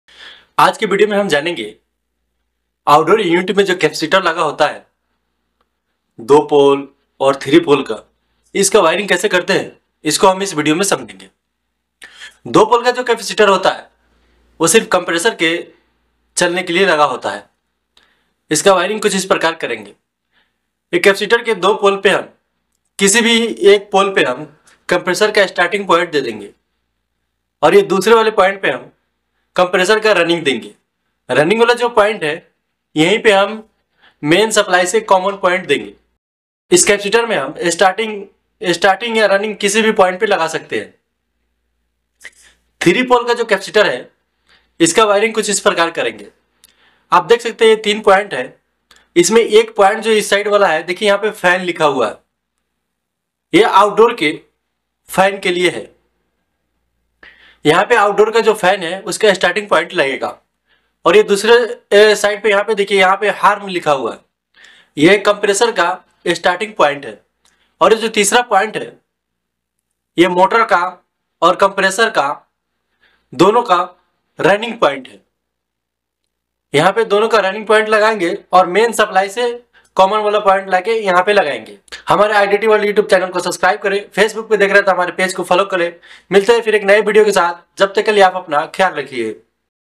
Two 2 poll Three poll 3 Capacitor Ka Wiring Kaise Kare/दो 2पोल या 3 पोल या का कैपेसिटर का वायरिंग/
Two 2 poll Three 3 poll Capacitor Ka Wiring Kaise Karen/
दो पल या 3 पोल का कैपेसिटर का वायरिंग कैसे करें/
इस वीडियो में बताया गया है कि आप capicitar का वायरिंग कैसे करते हैं पूरा वीडियो देखे हिन्दी/ऊर्दू ।idtworld
कंप्रेसर Start होते टाइम (हाई Amperes )क्यों लेता है इसका क्या-क्या रीजन है इस वीडियो में बताया गया idtworld dtworld

Mere chainnl par app AC ripening ke bare me .or. coiling system ke bare me bataya. Jata hain. Air-conditioner